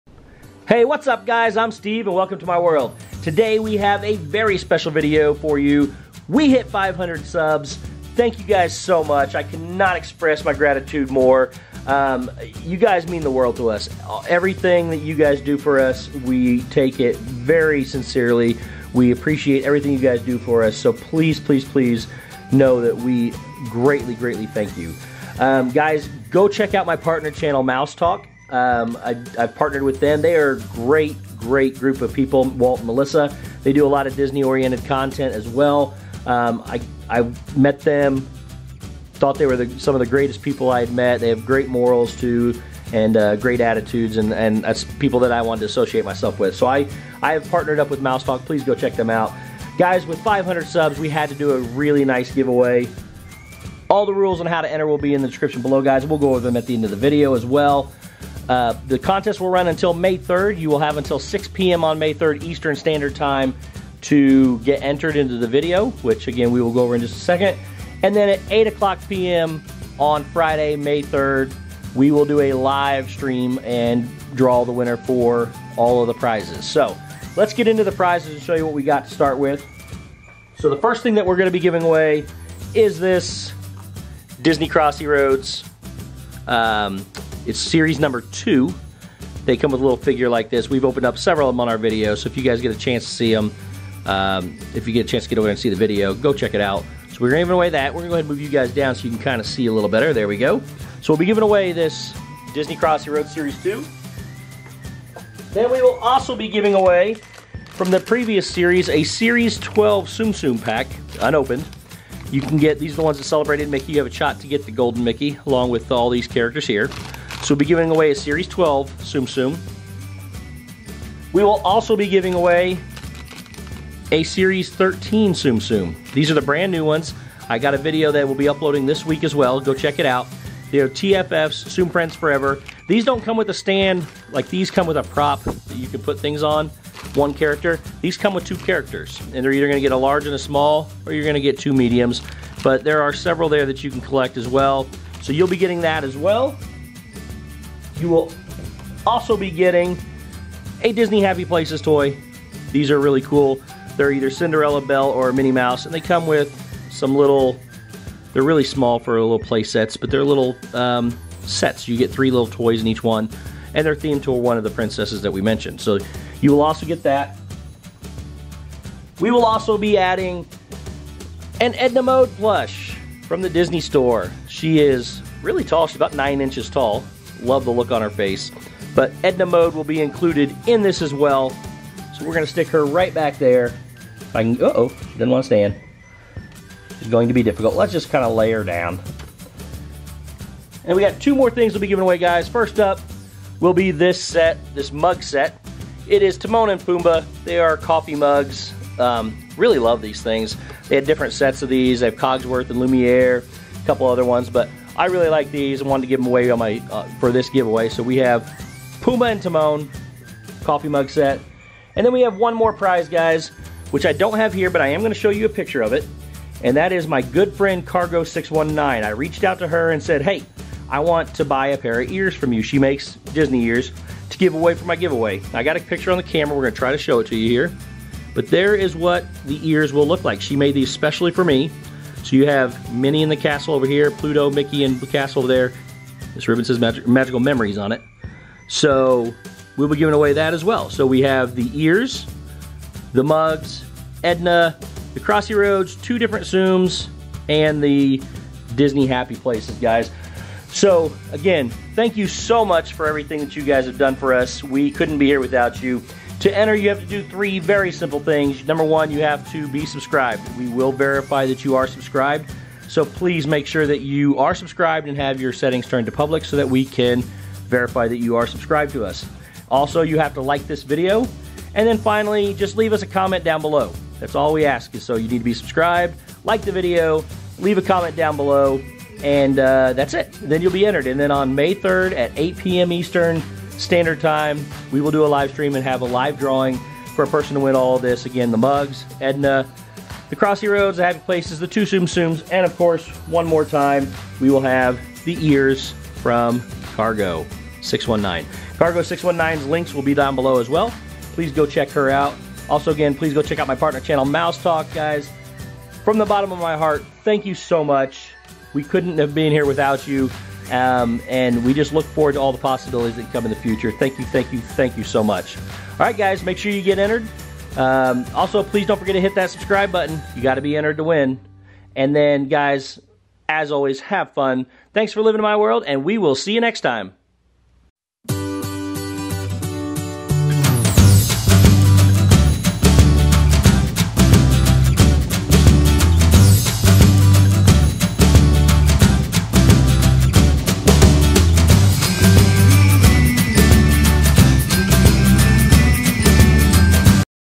500 Subscriber Giveaway! Thank you!
Thank you all so much from the bottom of my heart!

Th contest will run from now until Friday, May 3rd at 6:00 PM EST. We will live stream the results on May 3rd at 8:00 PM EST. There are three rules:

That is all! The winner will get everything from the video which includes:

1. Disney Crossy Roads Series 2 Figure
2. Disney Series 12 Tsum Tsum
3. Disney Series 13 Tsum Tsum
4. Disney Happy Places Blind Bag
5. Edna Mode Plush From The Incredibles
6. Timon and Pumbaa Mug Set
7. Disney Ears From Cargo 619
8. Disney T-shirt (XL) From Cargo 619

Again, I truly appreciate each and every one of you. I have made some really good friendships along the way and look forward to many more. Thank you!